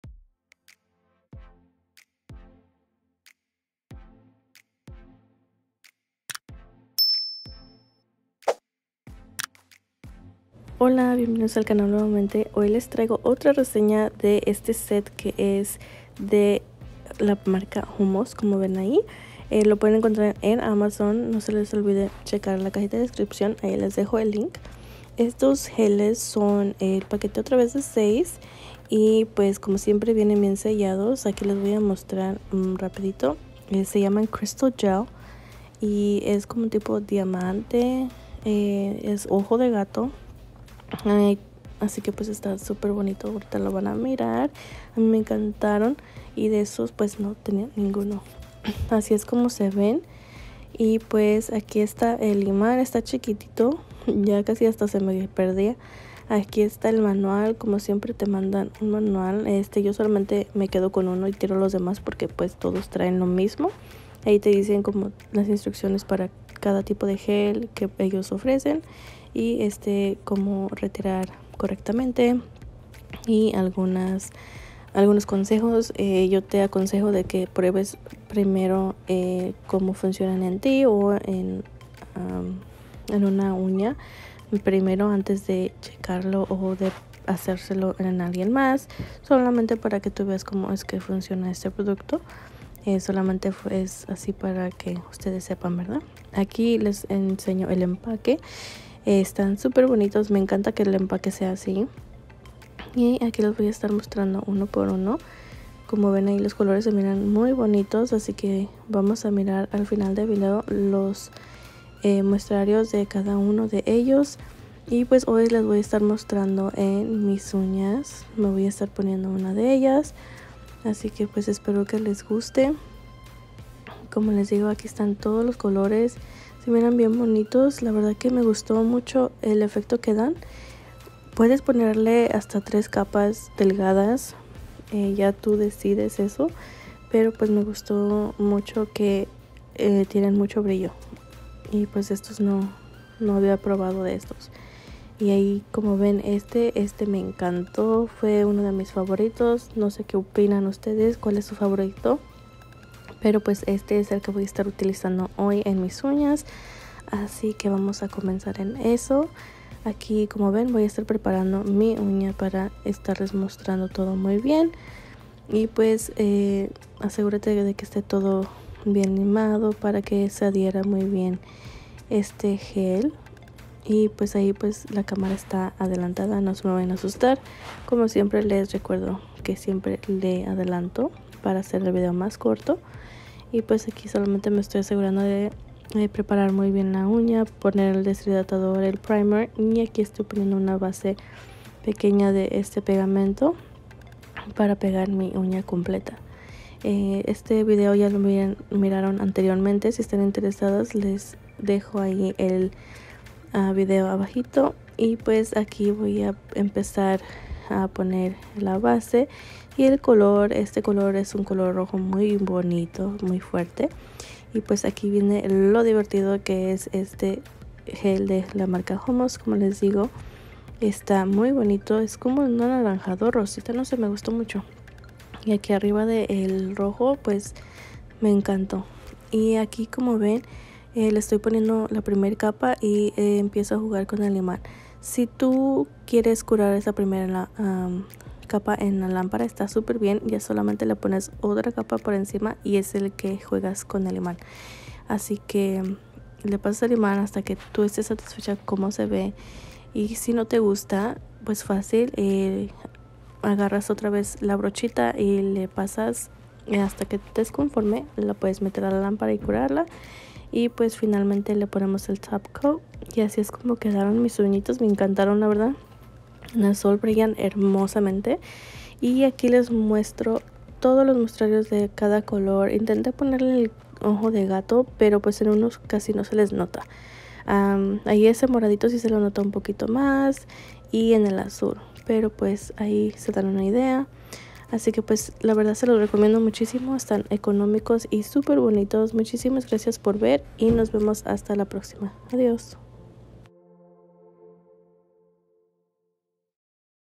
HOMOST gel ojo de gato adiamantado
⬇️👇🏻ABRE CAJITA DE INFORMACIÓN👇🏻⬇️

HOMOST Crystal Diamond Cat Eye Gel Nail Polish 6 Colors Set, Sparkle Shiny Gem Effect Gel Nail Polish Kit with Magnet

Xoxo, Angelica😘

BornPrettyStore.com
Usa código para 10% en tu compra de precio completo: Gelw10